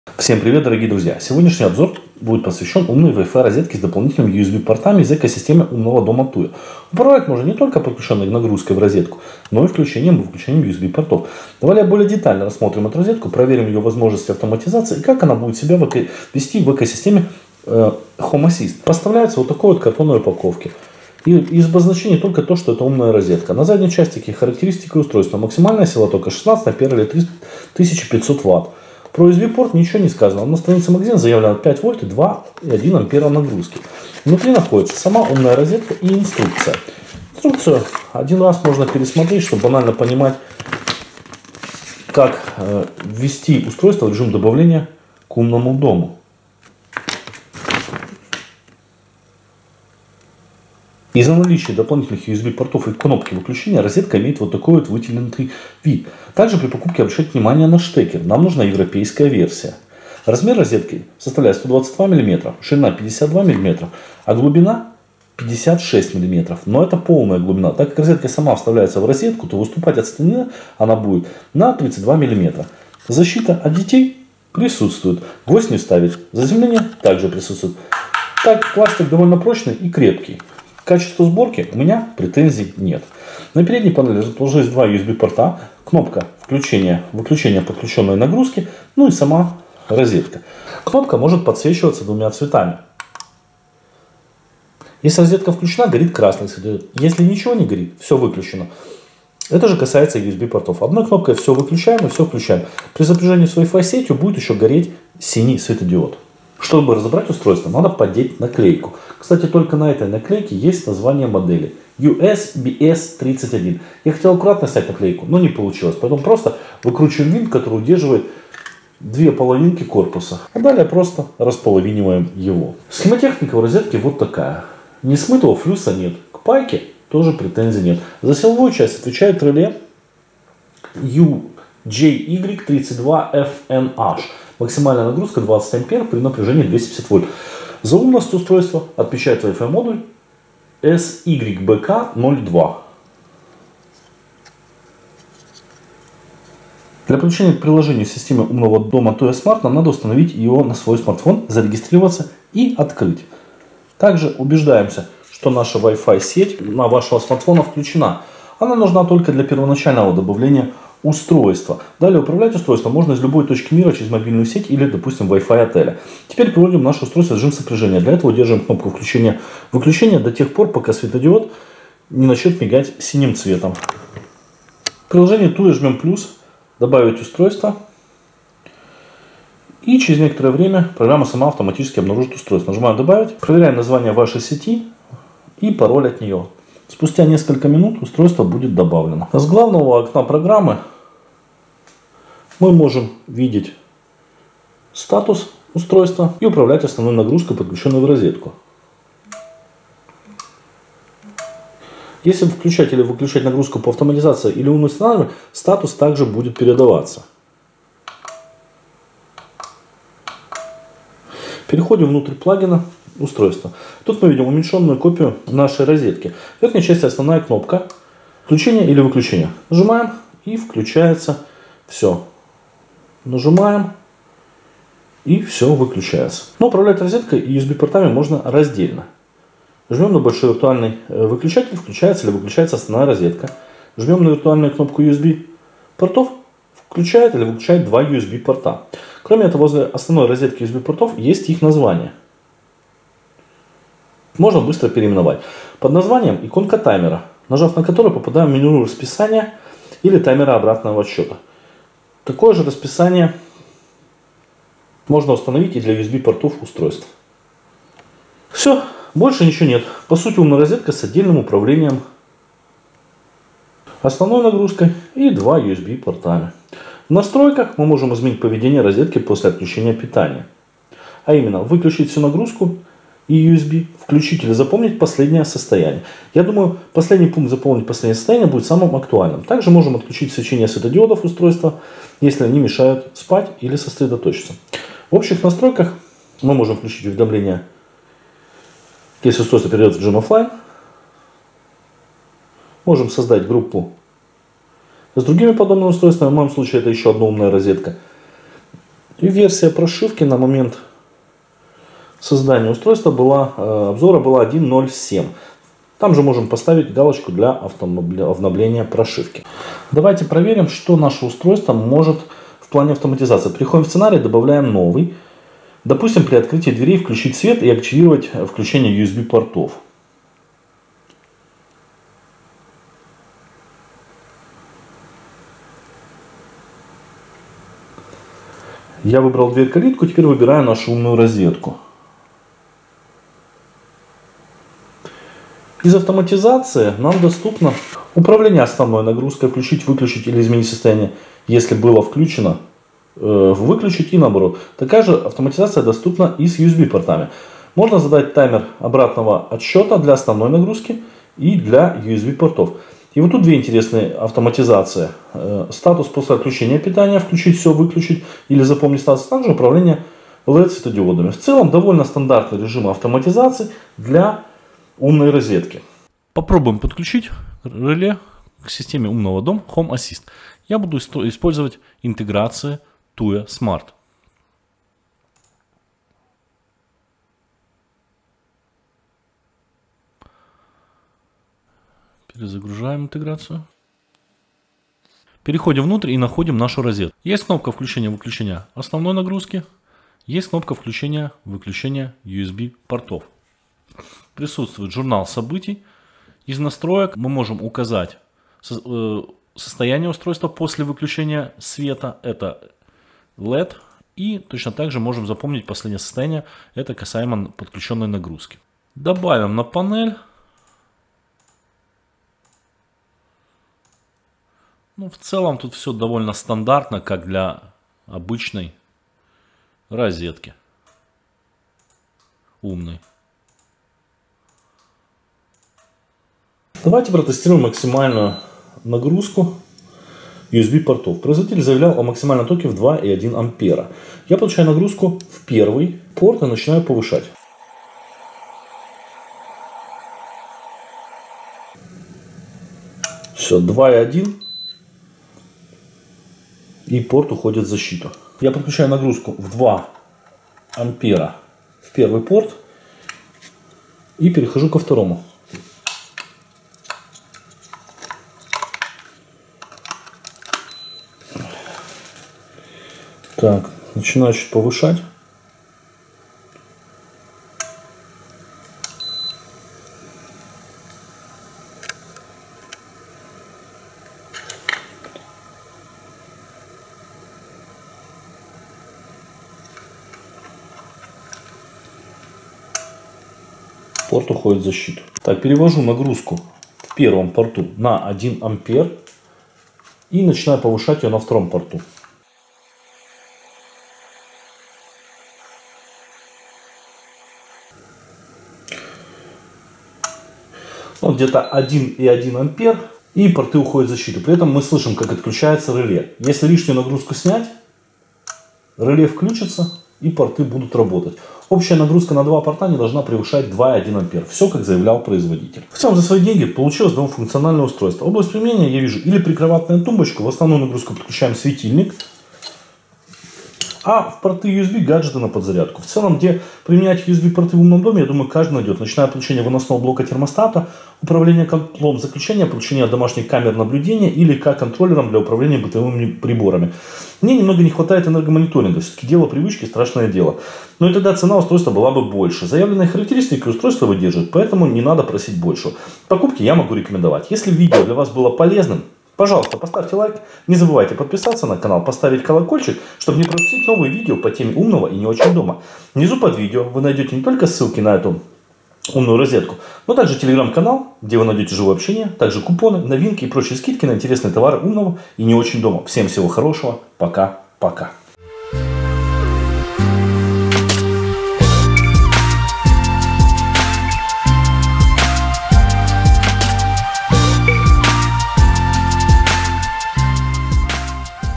Умная WiFi розетка IsFriday 16A с управляемыми USB портами 2А: удобство 2 в 1!
Обзор умной WiFi розетки IsFriday 16A с дополнительными USB портами из экосистемы умного дома TUYA. Это устройство позволяет не только удаленно управлять подключенными устройствами, но и контролировать работу USB портов со смартфона. В этом видео подробно рассмотрим все особенности этой умной розетки, её возможности в автоматизации Tuya smart. Погрузимся в детали, проверим автоматизацию и рассмотрим взаимодействие с экосистемой Home Assistant.
🔌🔋  IsFriday 16A - это не просто умная розетка. Она предоставляет максимальную гибкость в управлении вашими электроприборами, поддерживает силу тока до 16 ампер и обеспечивая до 3500 Ватт энергопотребления. Что позволяет подключать довольно мощные при бору и управлять ими удаленно или через расписание, или автоматизации.  Но это еще не все - IsFriday оснащена двумя дополнительными USB портами (5 Вольт и 2,1 ампер) что позволяет не только заряжать ваши гаджеты, или подключать различные приборы умного дома, но и управлять ими вне зависимости от основной нагрузки. При этом используя всего одно гнездо розетки!
Интегрированная с экосистемой Tuya Smart, IsFriday 16A легко управляется через ваш смартфон, давая полный контроль над подключенными устройствами даже издалека. А совместимость с Home Assistant открывает бесконечные возможности для автоматизации и интеграции в ваш умный дом.
От домашнего офиса до спальни - эта умная розетка прекрасно впишется в любое место вашего дома, обеспечивая надежное и интеллектуальное управление вашей техникой. Позвольте IsFriday 16A сделать вашу жизнь проще и удобнее уже сегодня!
Не упустите возможность узнать больше о возможностях умной розетки IsFriday 16A от Tuya Smart - вашего верного помощника в умном доме!

00:00 – 00:18 Вступление
00:18 – 00:49 Комплектация
00:49 – 01:46 Внешний вид
01:46 – 02:26 Что внутри
02:26 – 05:09 Работа в TUYA SMART
05:09 – 06:06 Пример автоматизации
06:06 – 07:06 Работа в HOME ASSISTANT
07:06 – 08:35 Тест портов
08:35– 09:40 Выводы